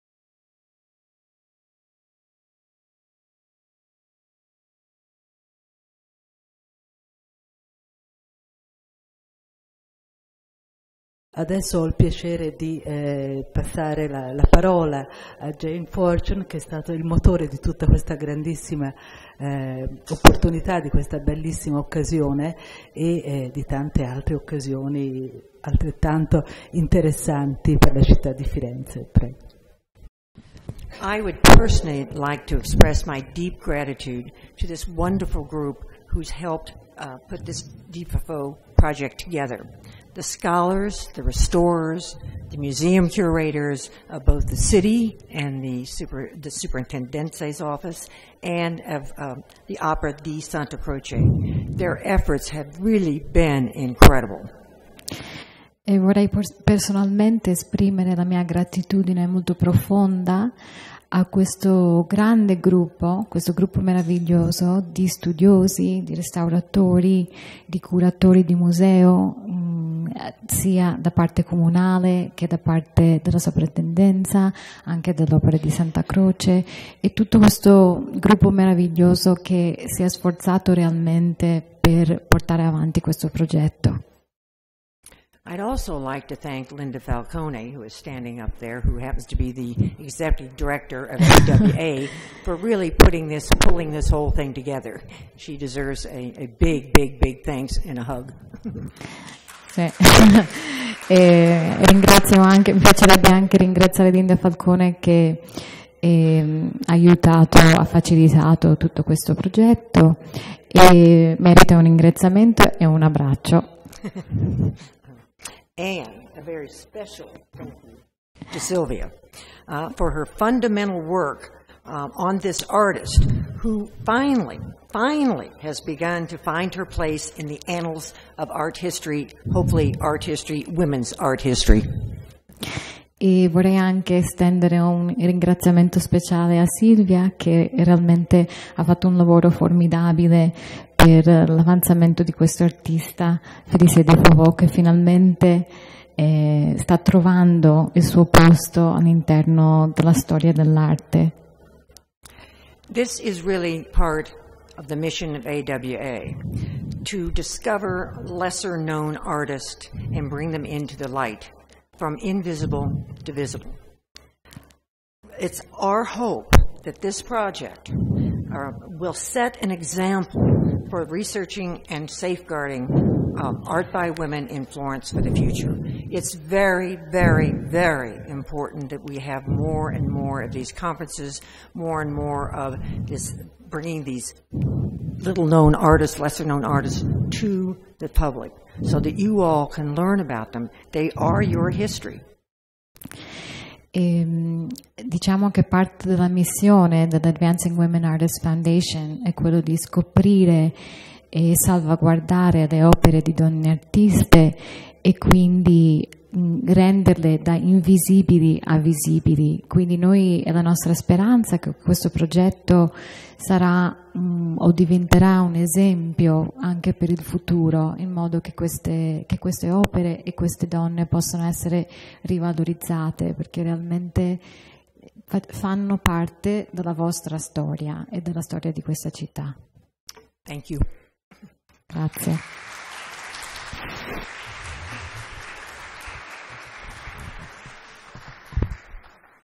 AWA Founder Jane Fortune speaks about Santa Croce Restoration
Jane Fortune, Founder and Chair of the Advancing Women Artists Foundation (AWA) gives a welcome speech during conference on French sculptress Fèlicie de Fauveau. AWA funded the restoration of the artist's marble monument in Santa Croce honoring poetess Louise Favreau.